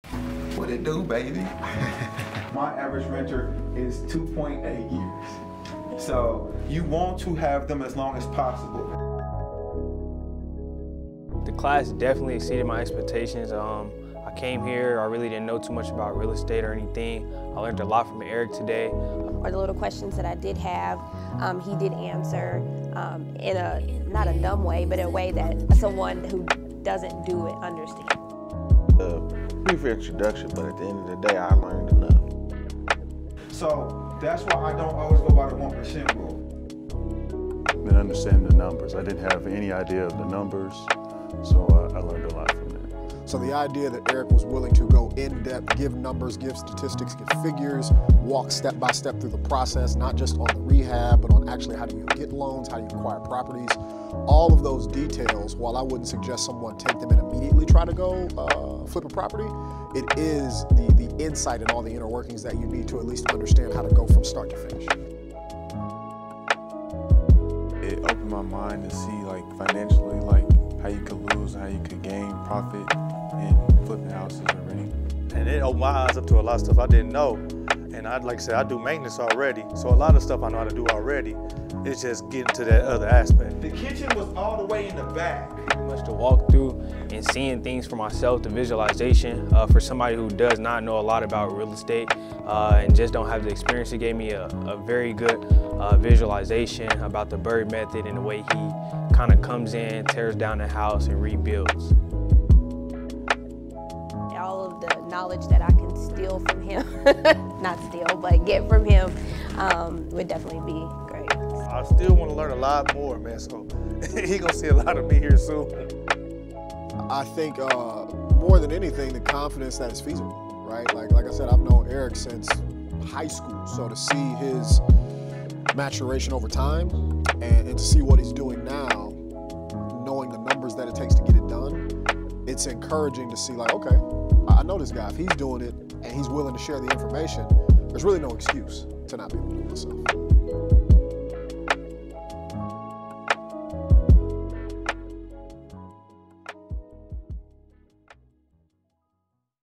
Jefferson Walkthrough
In this video, Erick Armstrong host his weekly Repair Estimate class with potential students. During the walkthrough of the property, Armstrong breaks down how to calculate the cost of repairs for a home. This provides a hands on experience for those wanting to learn more about a major proponent of real estate investment.